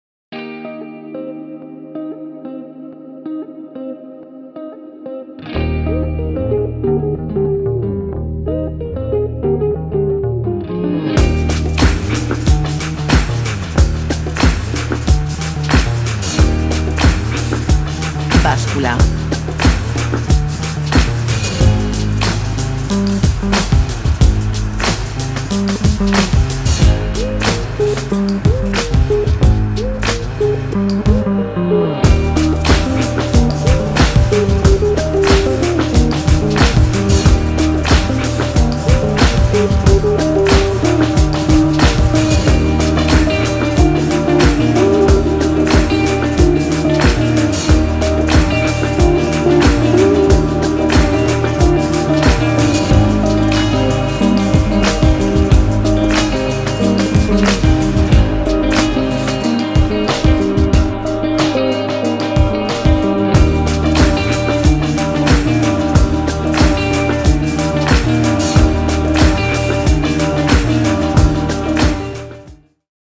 Descubre la Función Báscula de la Olla GM Modelo G Deluxe
Descubre la nueva función báscula de la Olla GM Modelo G Deluxe.